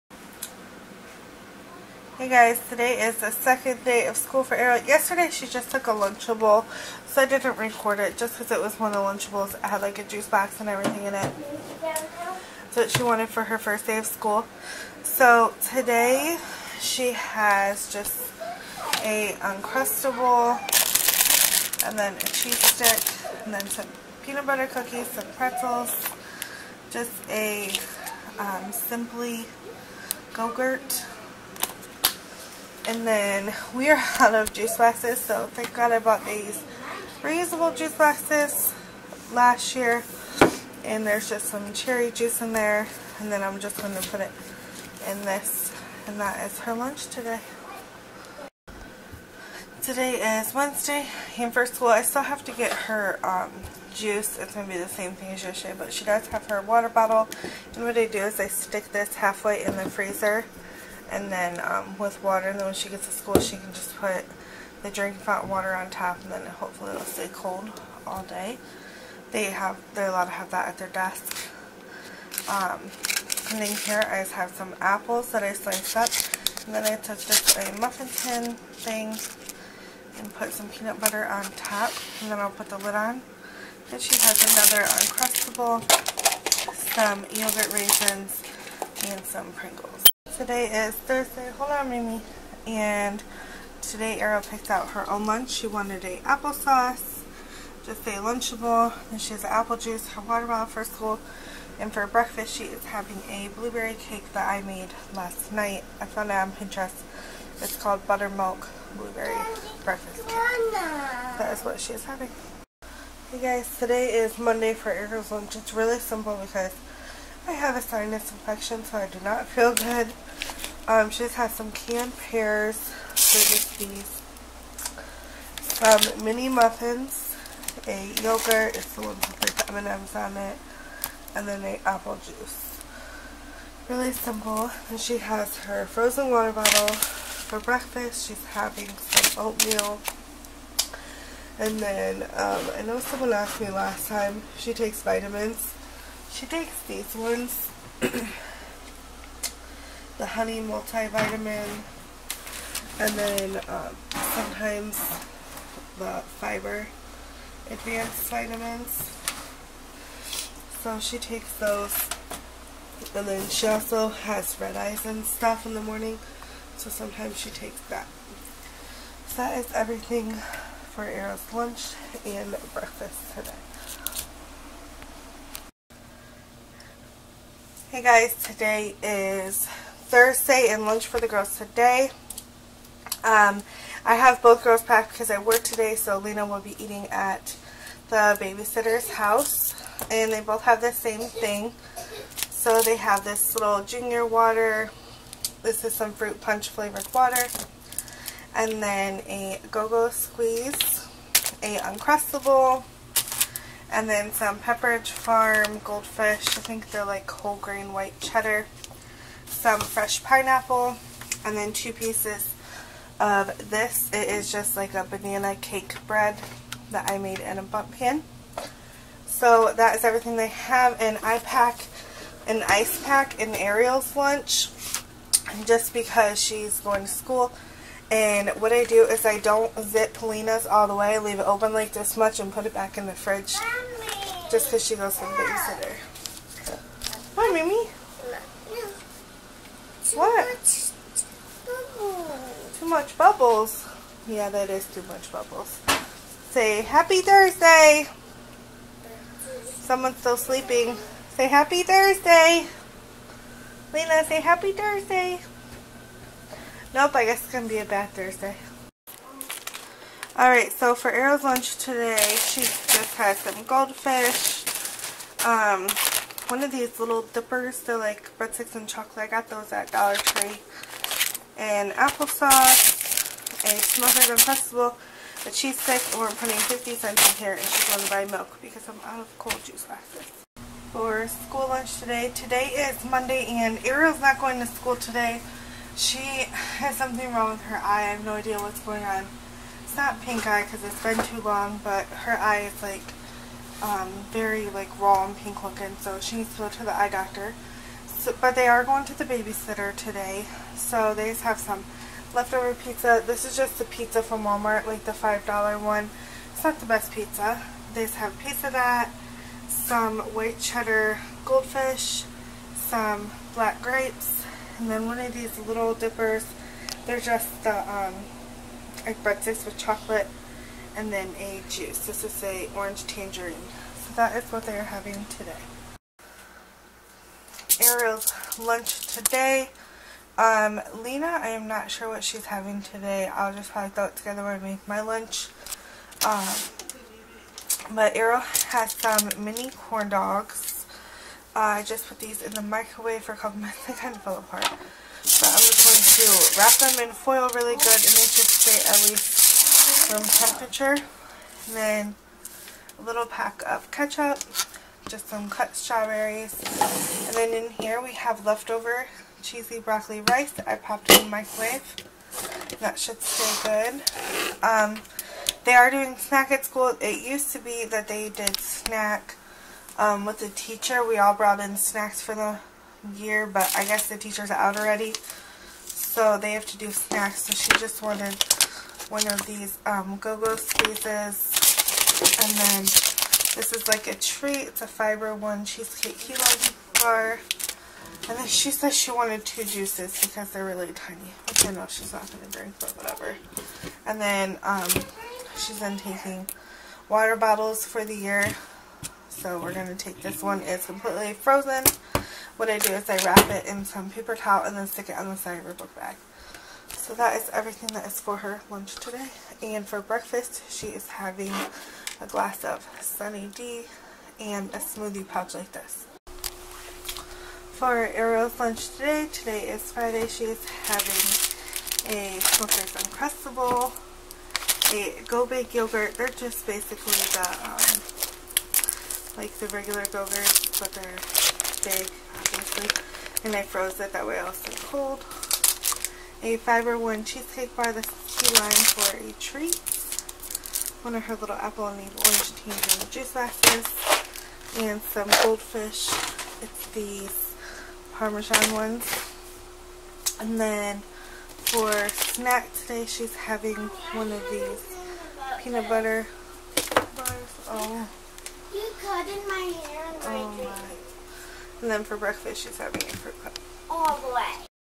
School Lunch
What I pack my daughter for her first grade lunch